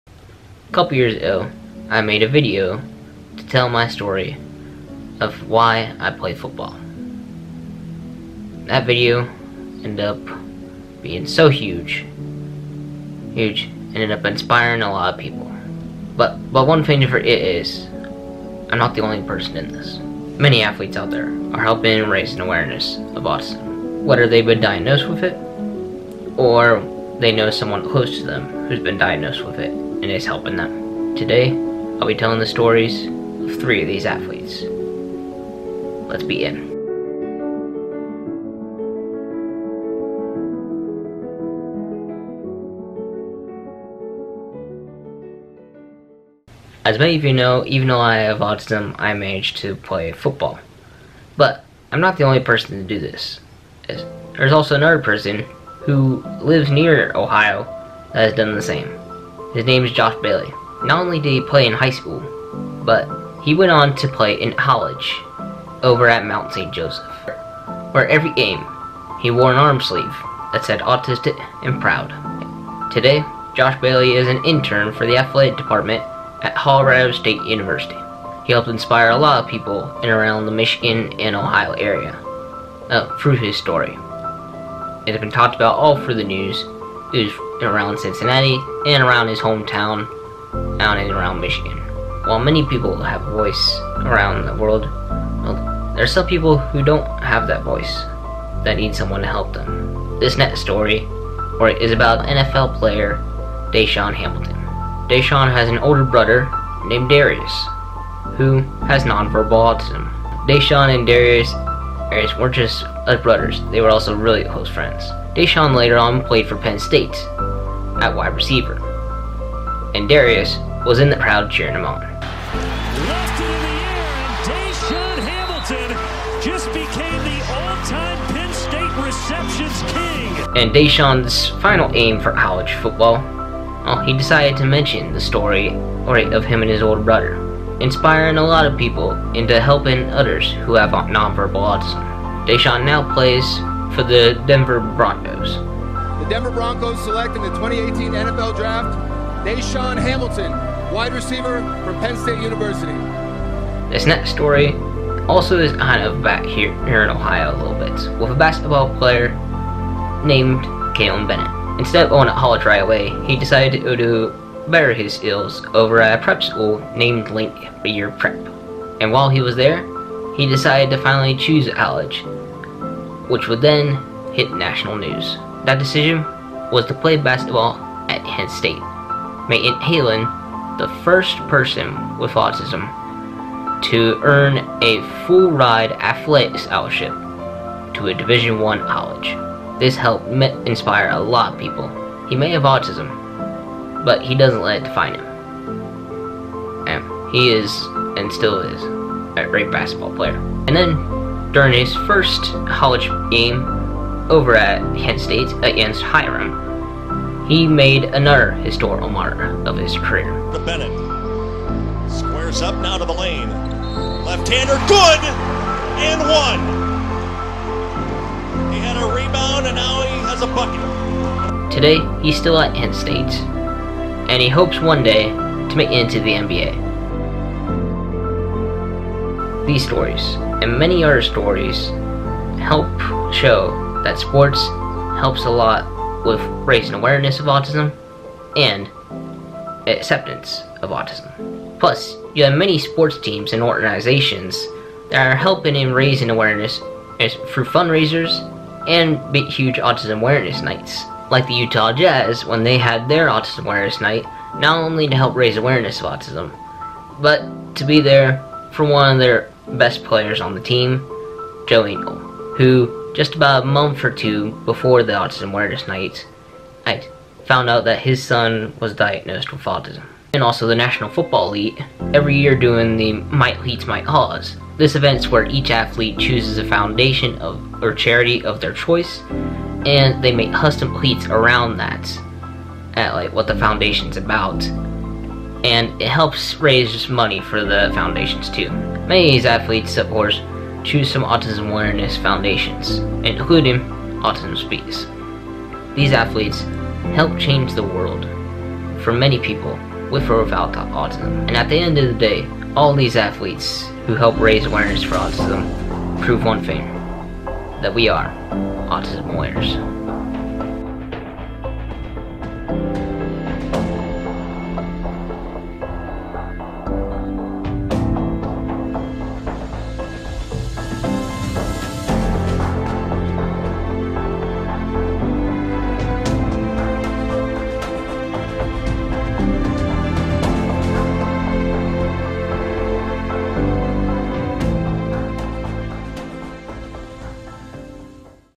'We Are Autism Warriors' Short Film
For Autism Awareness Month, I decided to do a short film telling the inspiring stories of three athletes from college to professional sports who've helped show how sports can help raise awareness and acceptance of autism. A couple of years ago, I made a video about the reasons why I played football and raising awareness of autism was one of those many reasons. I want to help share my story and these other athletes' stories to keep spreading awareness and acceptance because there are many other possible stories like ours to be made. We can all help and be Autism Warriors!

Maybe DROP a LIKE?

What about leave a comment??

LOL

SOCIAL MEDIA